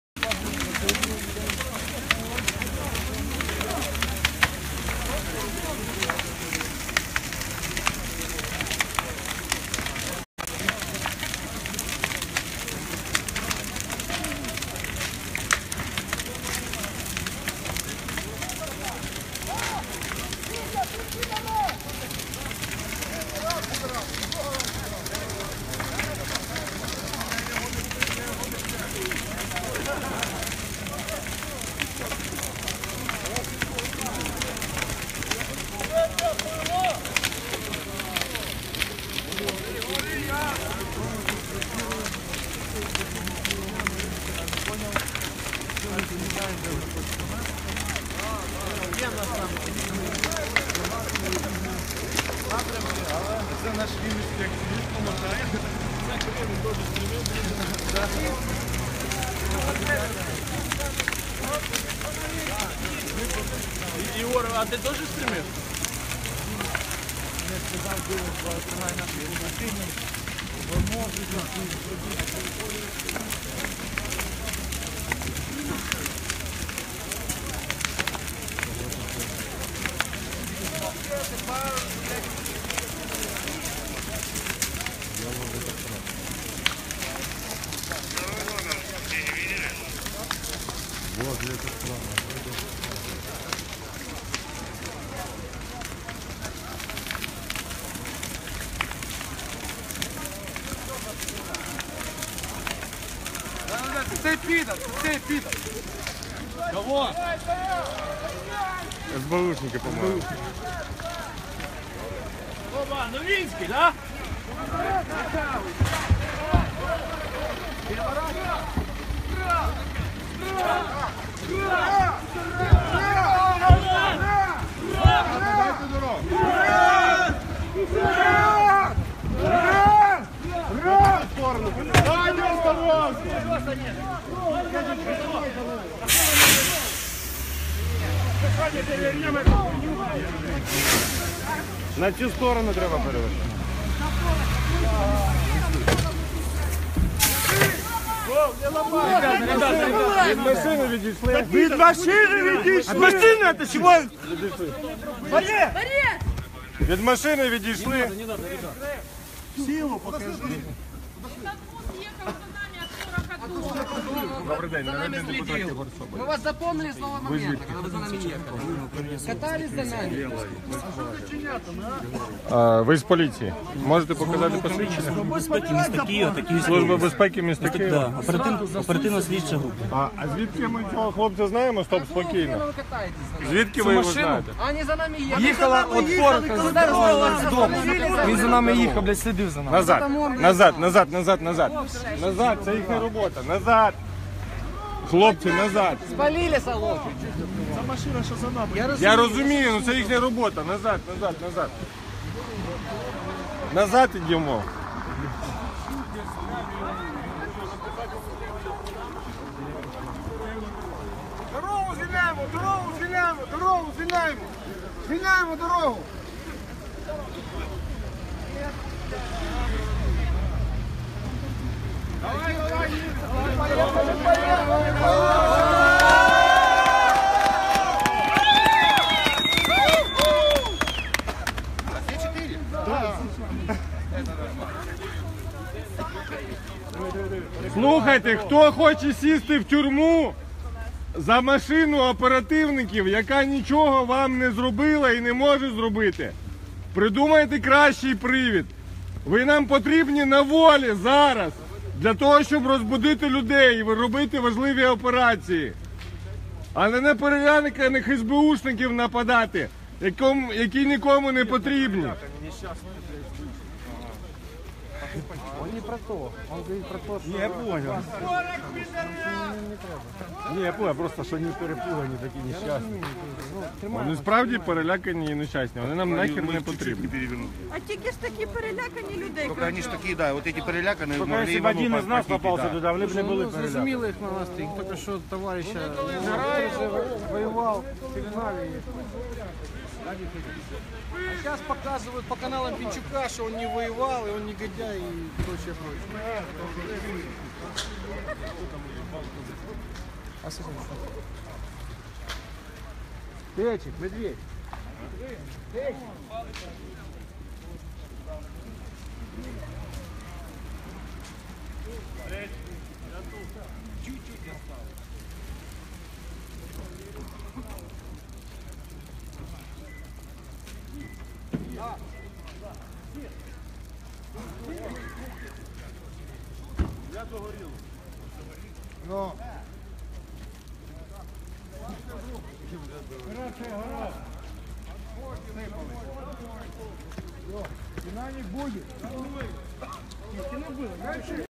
18.11.2017 Протести біля адміністраціі президента України на Банковій і машина СБУшників
ПАЛАЛА ШИНА!